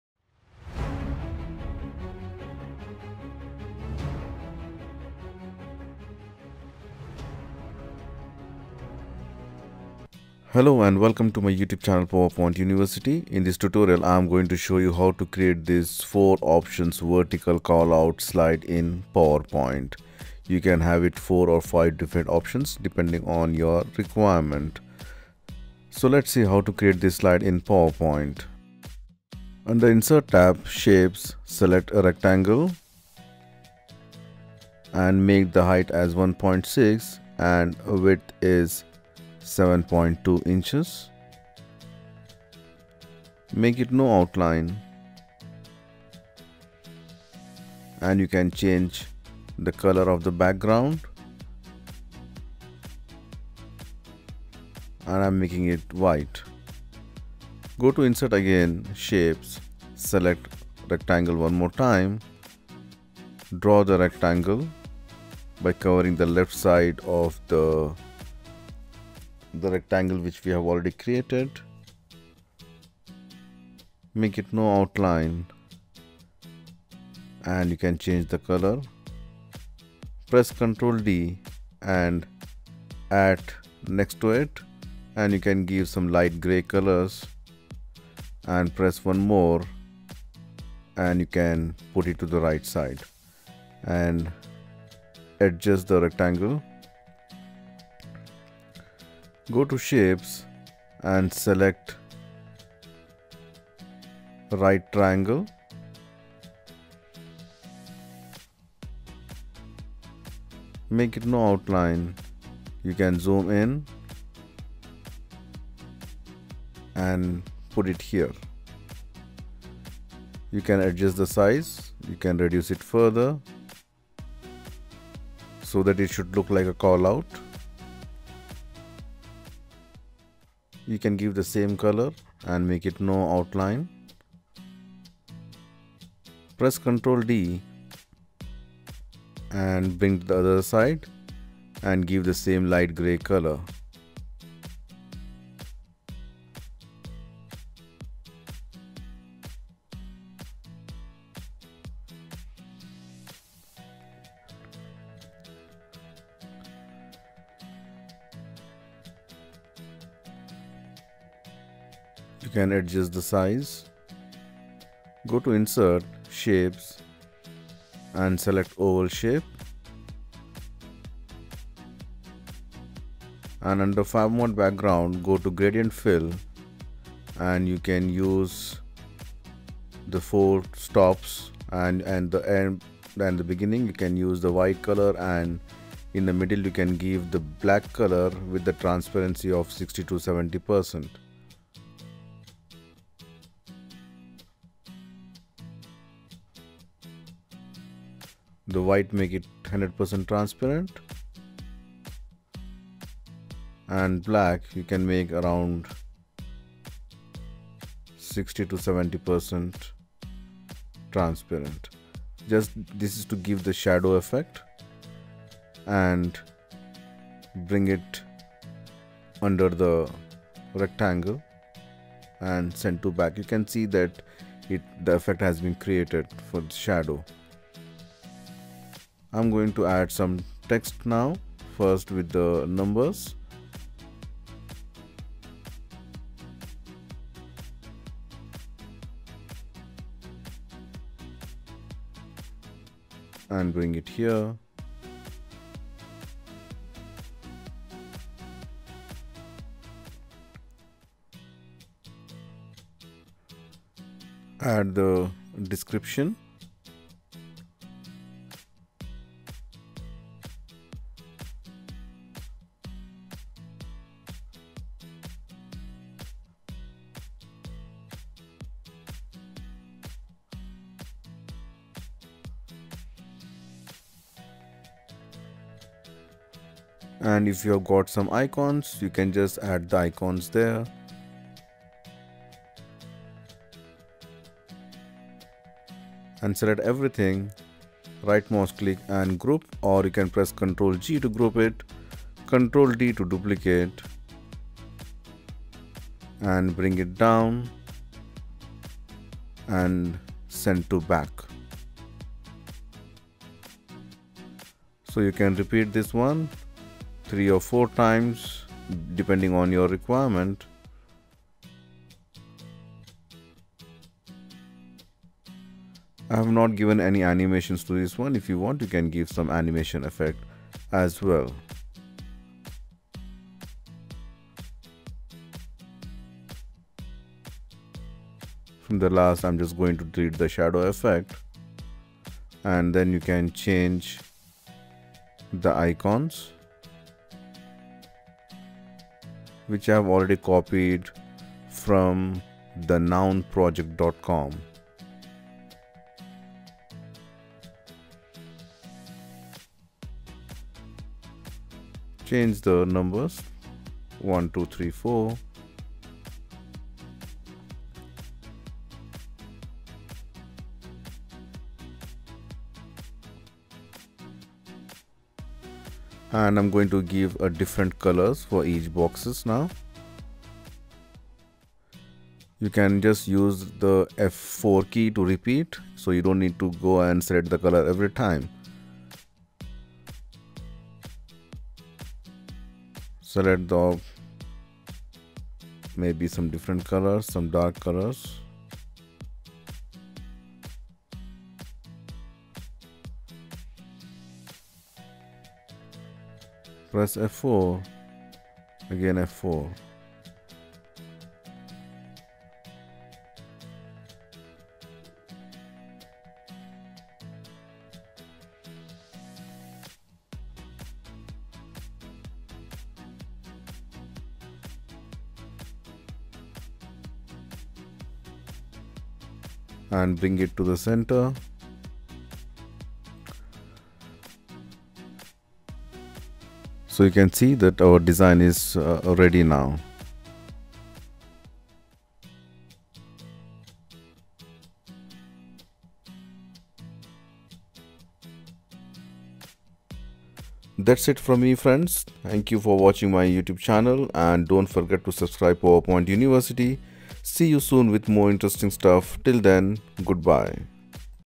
How to make simple designs in PowerPoint
In this tutorial, I have explained how to create a simple design with basic powerpoint shapes.